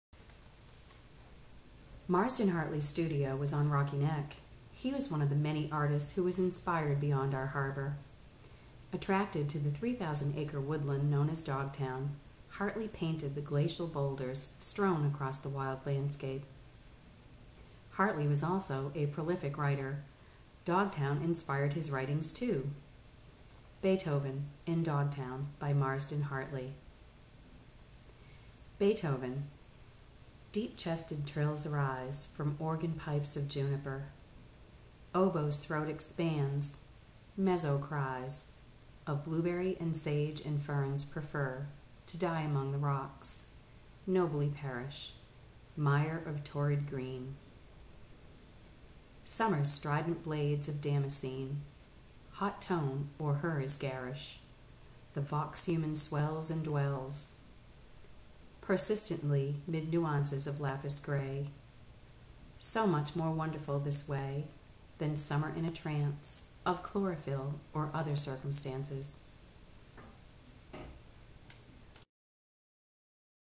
Gloucester HarborWalk #23: Marsden Hartley Dog Town Poem
Marsden Hartley poem Beethoven (in Dogtown). From Marsden Hartley inscription on the reverse of his painting Flaming Pool,Dogtown, 1931. Quoted in Poetry of Places in Essex County. North Shore Community College.